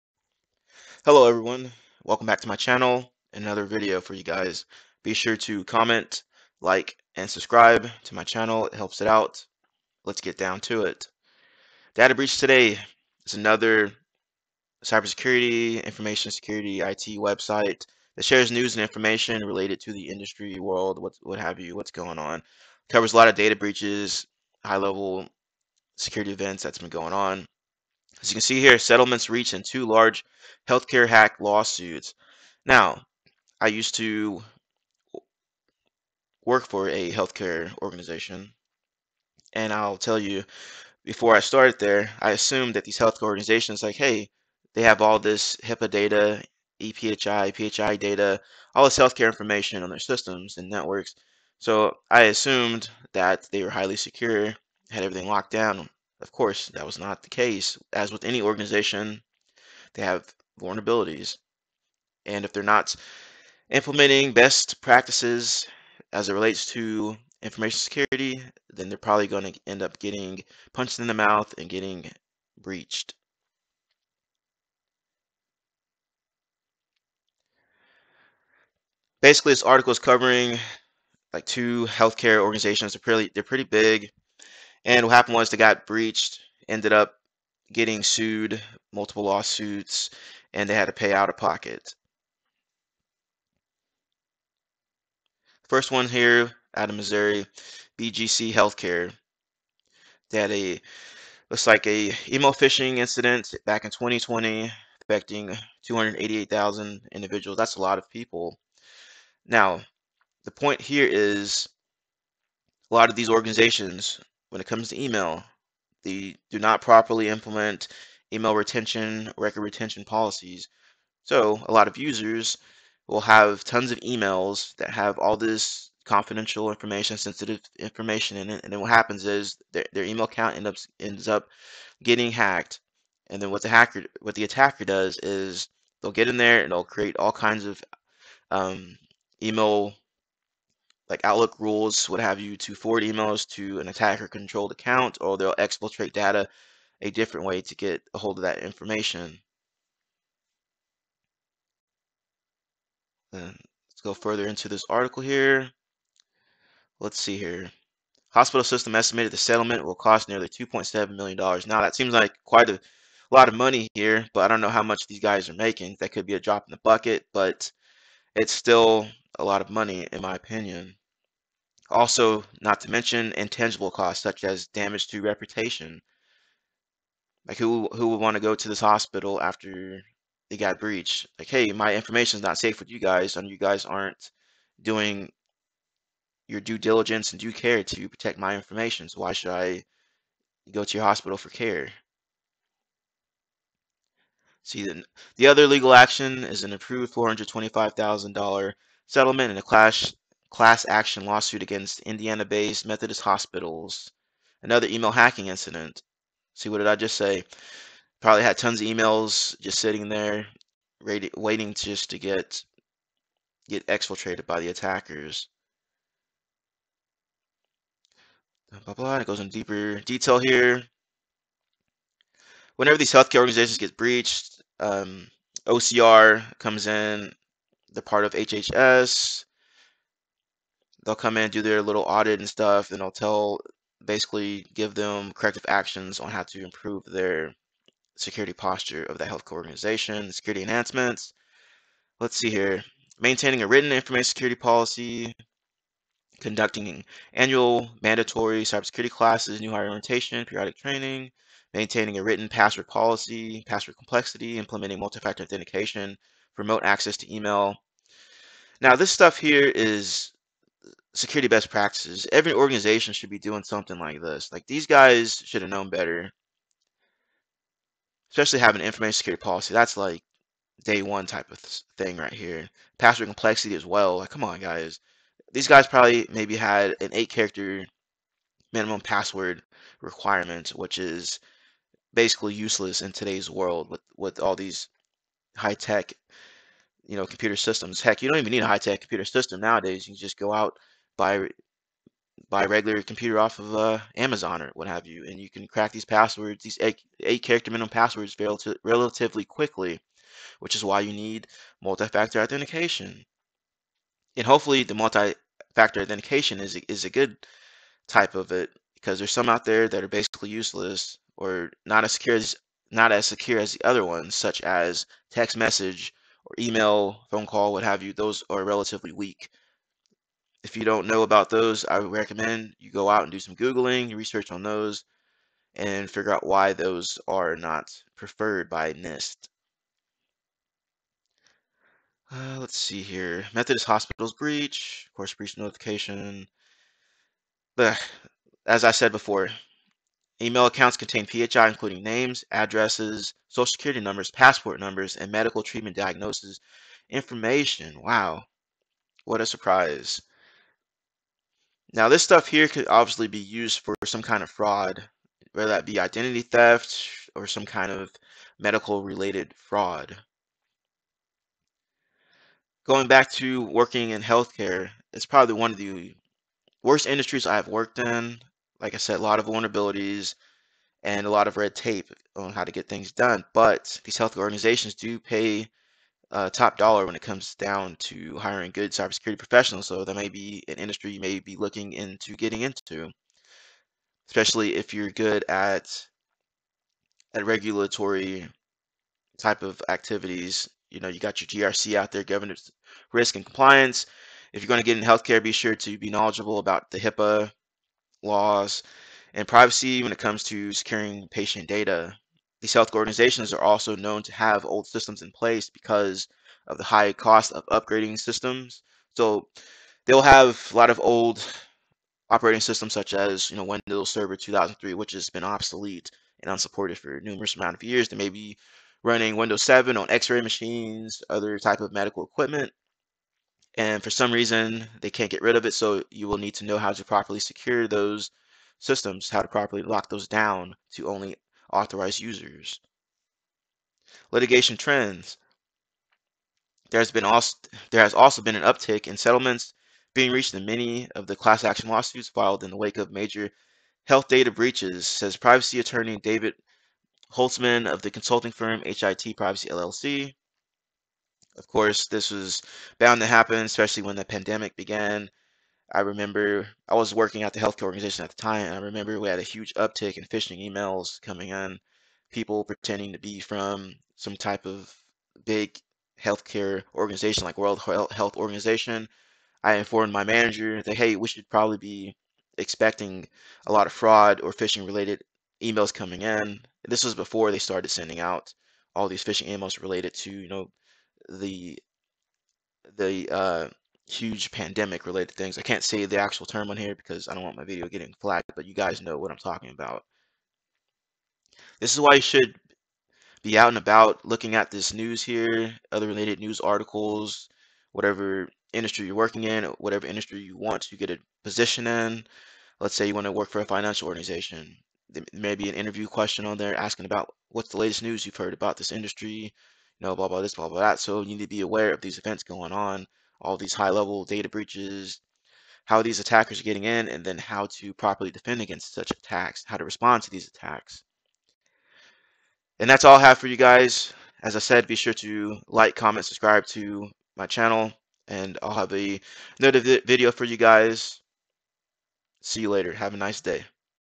Is the Doctor in the House? Healthcare Data Breach Lawsuits
Covering an article about recent healthcare data breach lawsuits.